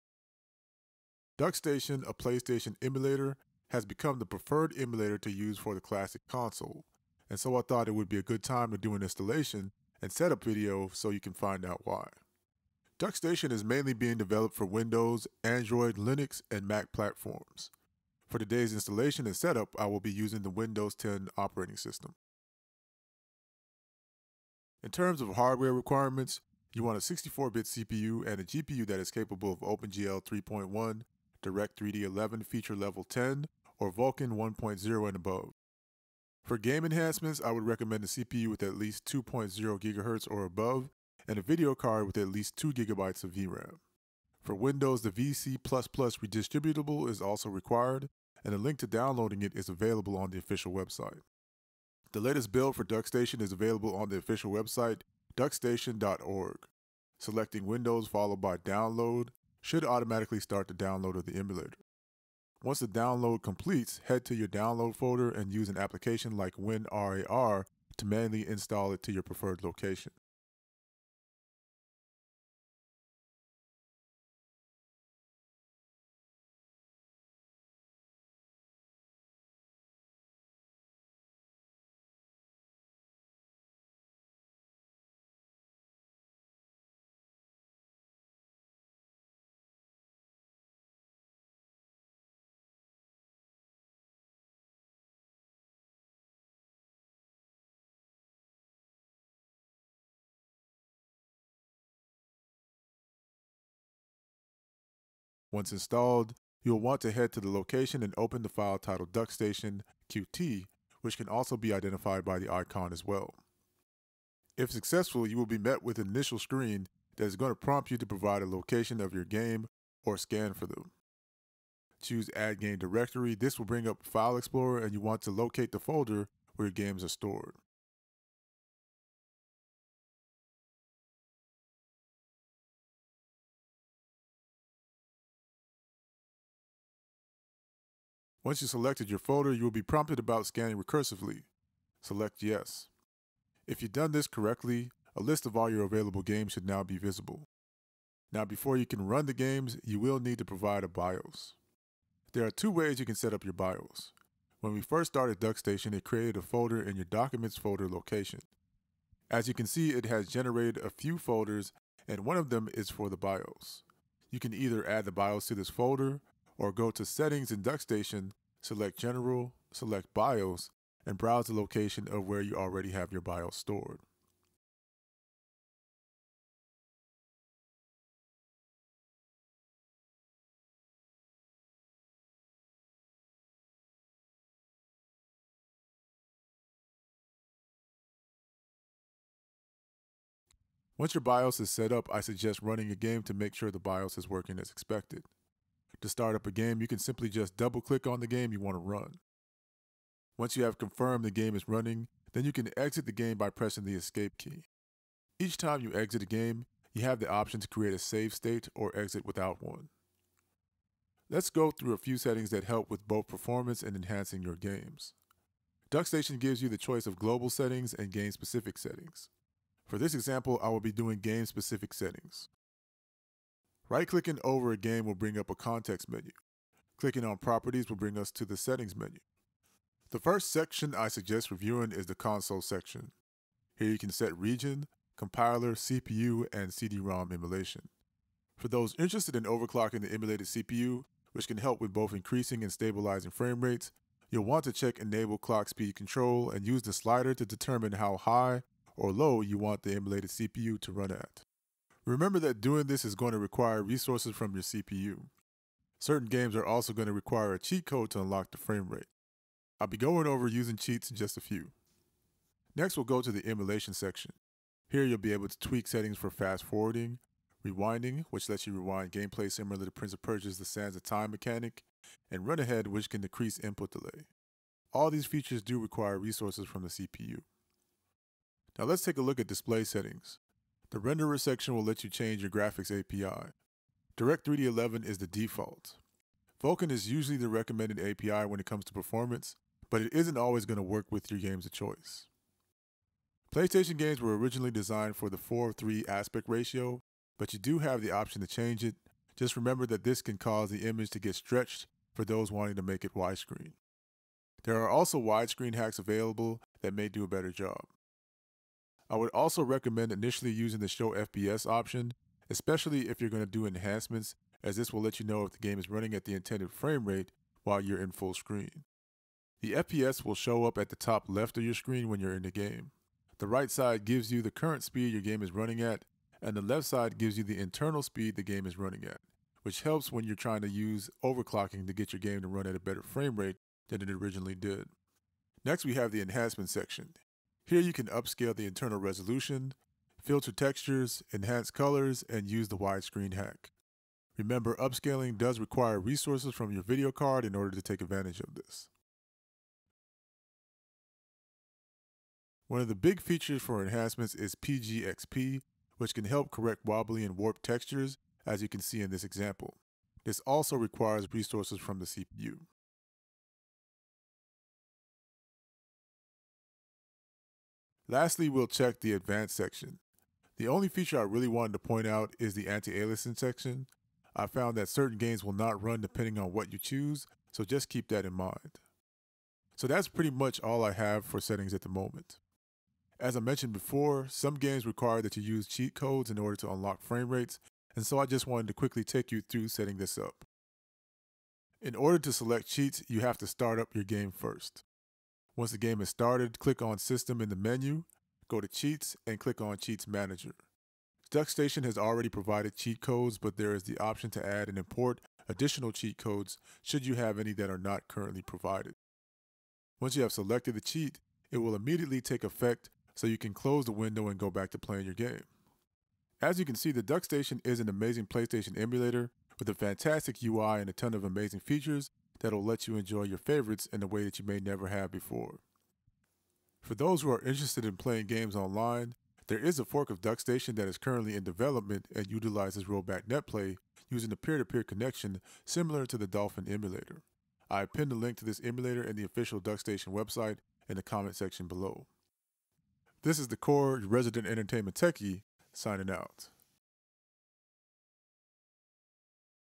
DuckStation "PlayStation 1 Emulator" 2023 Installation and Setup Guide
DuckStation has become the preferred way to emulate the original PlayStation console and so I created this installation and setup guide to help you get this emulator up and running.

In this guide we cover downloading, installation, and settings that will enhance your PS1 games.